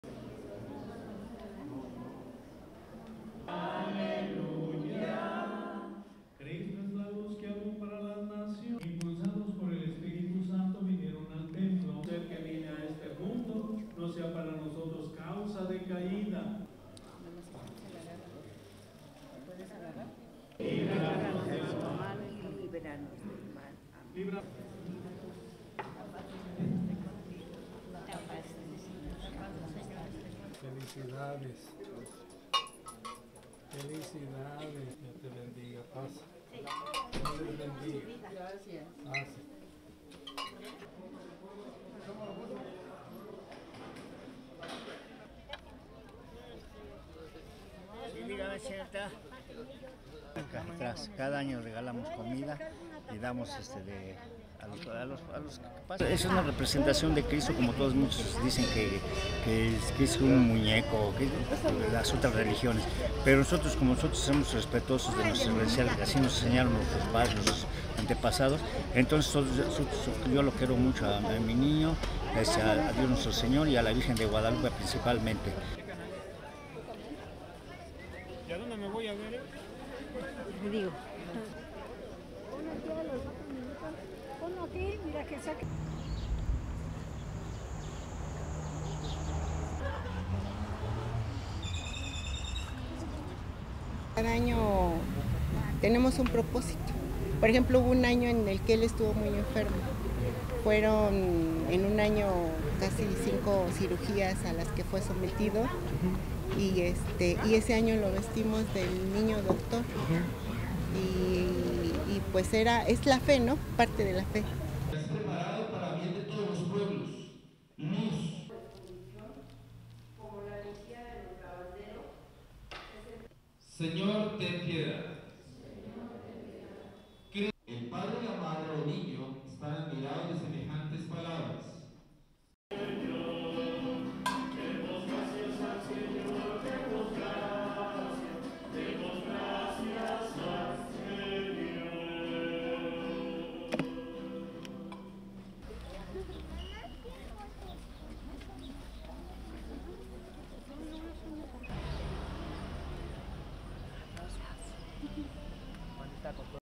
Mexico celebrates Candelaria virgin
(2 Feb 2020) Mexicans celebrated the day of the Candelaria virgin on Sunday, carrying small figures of baby Jesus to church where they received the priest's blessing for the rest of the year.
The festivity has its origins in Europe, where an apparition of the virgin in the Spanish Canary Island town of Tenerife led to the celebration every 2nd of February, was later on brought to the Americas and is celebrated in Mexico to this day.
On Sunday, hundreds of devotees carried their baby Jesus figures dressed in elaborate outfits.
Francisco Alcantara celebrated his 71st birthday by bringing a 110-year-old baby Jesus made of wood that he inherited from his wife's family. Alcantara and his wife come year after year, and they say they feel very blessed having their baby Jesus with them.
"He is a representation of Christ," he said holding on the baby Jesus dressed in white. "We are respectful of our beliefs, that's how our parents, our ancestors taught us, so I love my baby (Jesus)."
Lourdes Martinez has dressed her Jesus figurine as the archangel of love and asked for her marriage to be blessed.
Last year, she dressed her baby Jesus as a doctor.
"Each year we have a purpose. For example, there was a year when he (referring to her husband) was very sick. He had five surgeries in one year. And that year, we dressed him (Jesus) up as a baby doctor," she said.